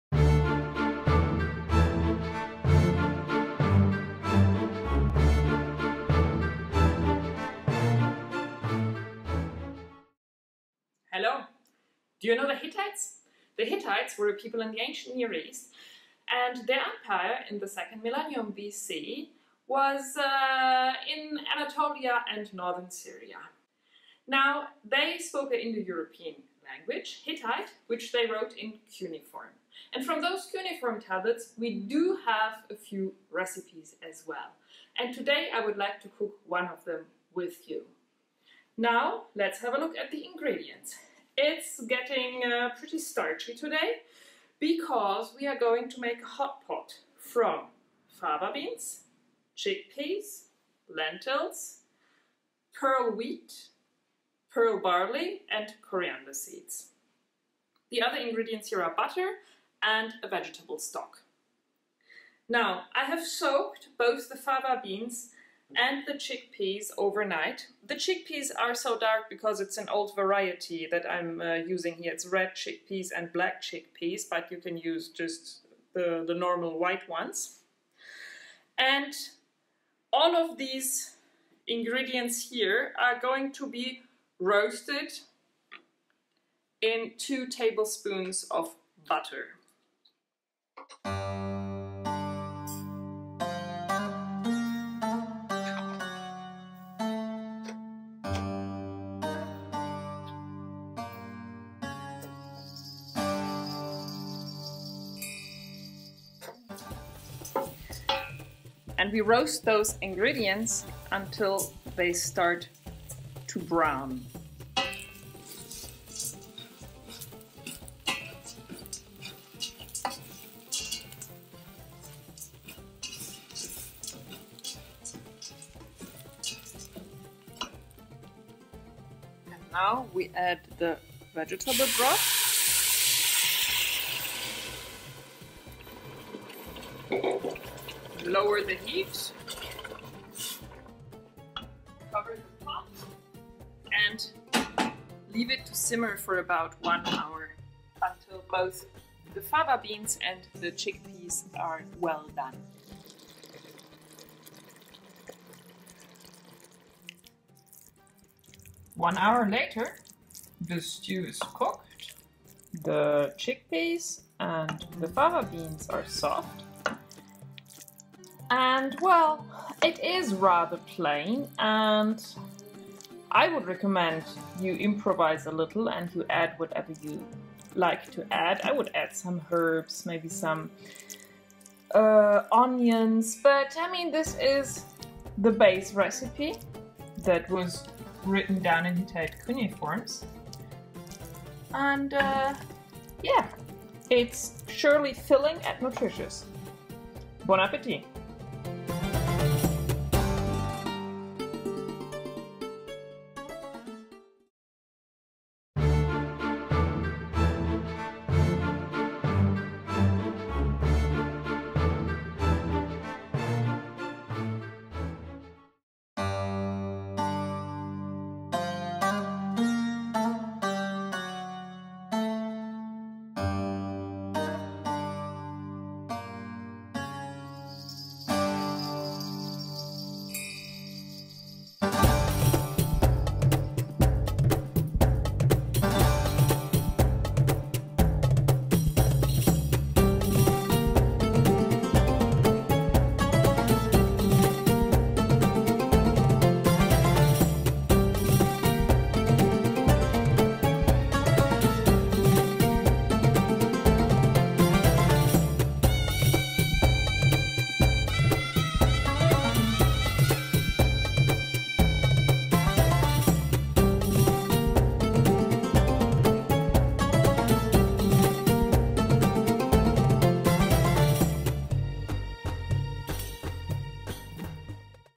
Hittite Hotpot - A Recipe from the 2nd millennium BC
The Hittites were a people that lived in the area of Anatolia and Northern Syria in the 2nd millennium BC. The Hittite empire existed from roughly 1,600-1,200 BC. (not counting the Neo-Hittite kingdoms of the early 1st century BC). Before the rediscovery of Hittite cities in what is now Turkey, Hittites were only known from the Bible. The Bible, though, is not very Hittite-friendly, although this civilization was extremely tolerant and in many ways quite advanced for its time. Given the geography, it is very likely that the Hittites in the Bible do not refer to the inhabitants of the Hittite Empire at all, but to Hurrian tribes in the Levant, which were also called Hatti. Anyway, here is the recipe of an original Hittite grain stew, translated from a cuneiform tablet from the Hittite capital Hattusa (Keilschrifttexte aus Bogazköy, 4.2 i 9-11):

You need: ½ cup dried, peeled broad beans, soaked overnight (or 1 cup already soaked and cooked)
½ cup dried chickpeas, soaked overnight (or 1 cup already soaked and cooked)
½ cup lentils
½ cup pearl wheat (or pearl spelt, if available)
½ cup pearl barley
1 tablespoon coriander seeds
2 tablespoons butter
4 cups vegetable stock

It is a rather plain porridge, hence I’m suggesting you consider it a base recipe and add some herbs and spices, maybe some fried onions and garlic. The original version doesn’t even mention stock but tells us to add water only. I’ve added the stock to make the dish more palatable.
This recipe is also featured in my new, upcoming cookbook, “From Eden to Jerusalem: Recipes from the Time of the Bible“.